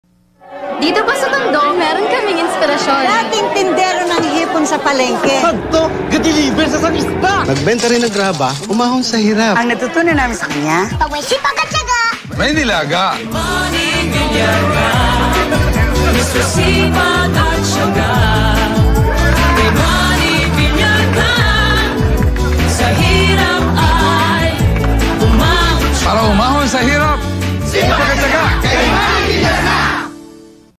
Manny Villar - Senatorial Ad (2007)
Manny Villar used his senatorial campaign in 2007 as the test bed for the theme that would define his presidential run a couple of years later. In this ad, Villar paints himself as once a pauper that's one with the commonfolks. And got to give credit where credit is due, that jingle is one catchy earworm.